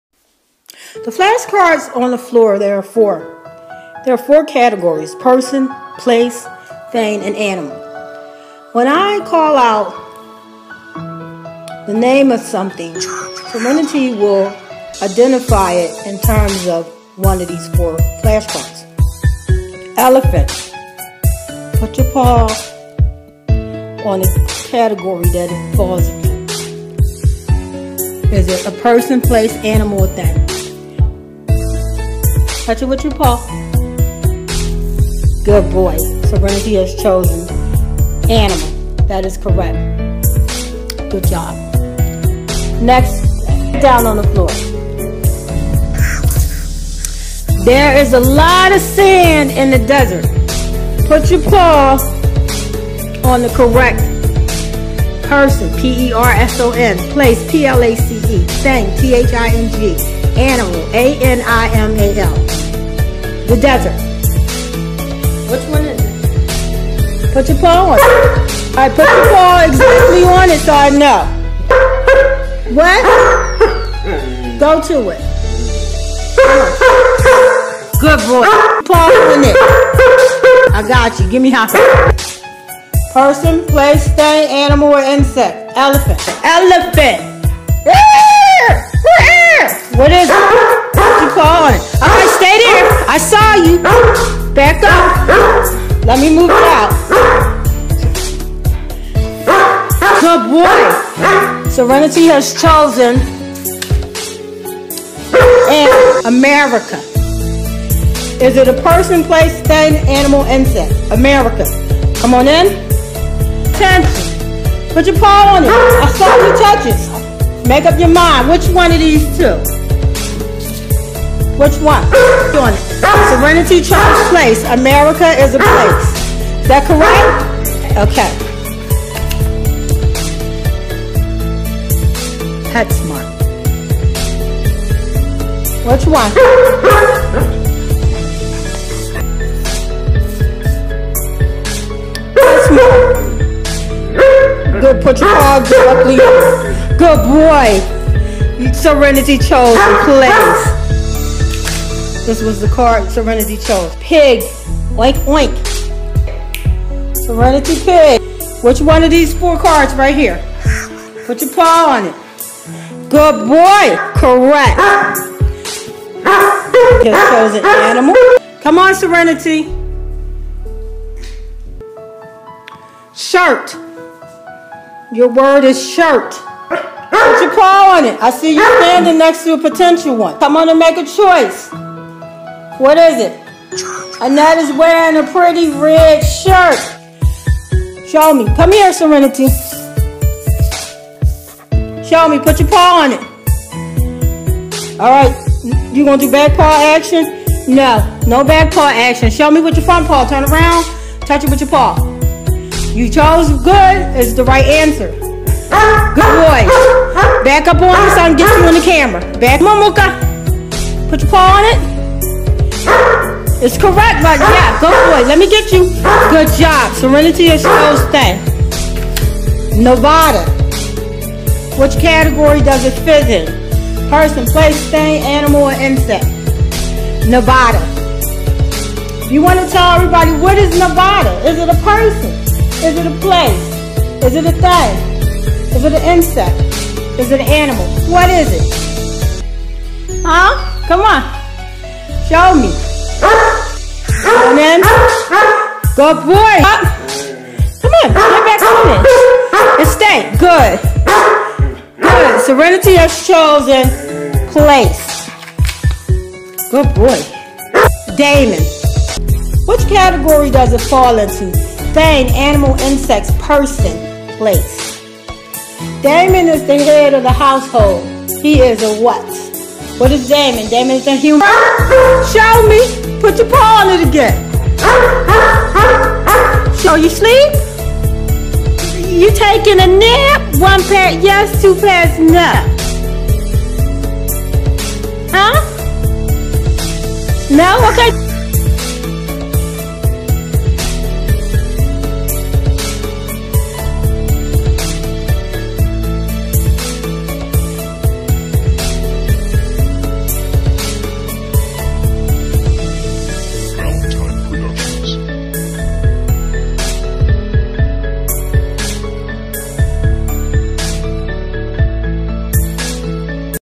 Collie Serenity Peace Discerns Person , Place, or Thing
Serenity will identify whether a word fits into the categories , of person, place, or thing.